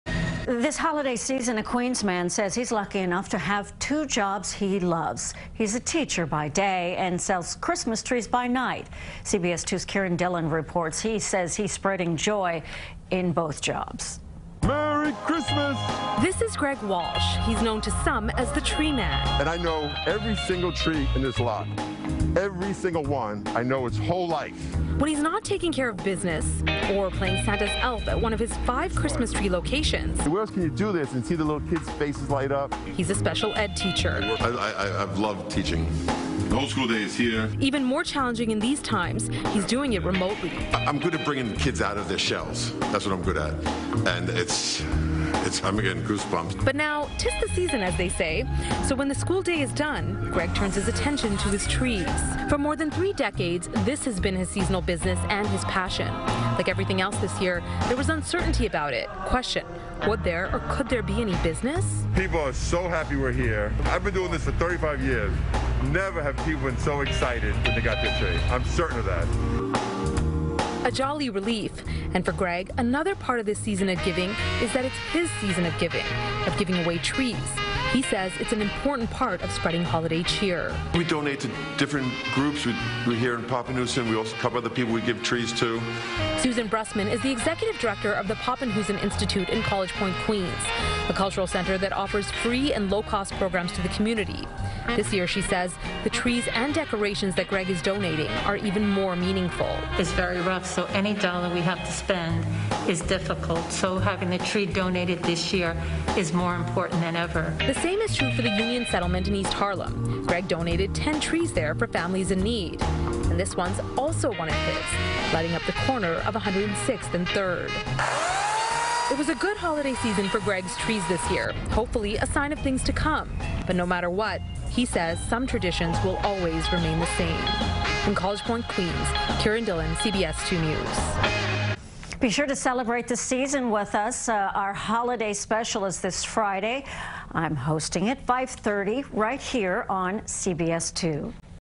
Queens Man Spreading Joy In Jobs As Special Education Teacher, Christmas Tree Seller
This holiday season, a Queens man says he is lucky enough to have two jobs he loves. He is a teacher by day and sells Christmas trees by night; CBS2's Kiran Dhillon reports.